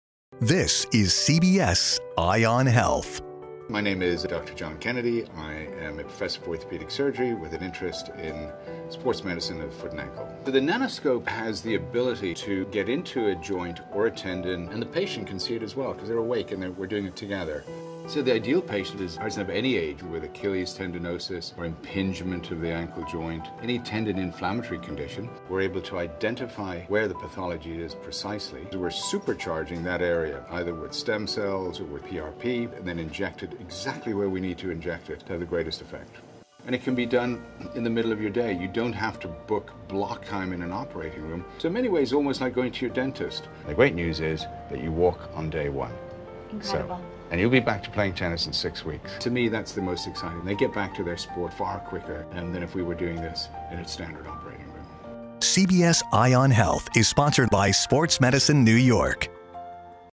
Benefits of the Nanoscope
Dr. Kennedy explains the use of the Nanoscope in accurate diagnosis and treatment of foot and ankle conditions.

Watch now to learn more!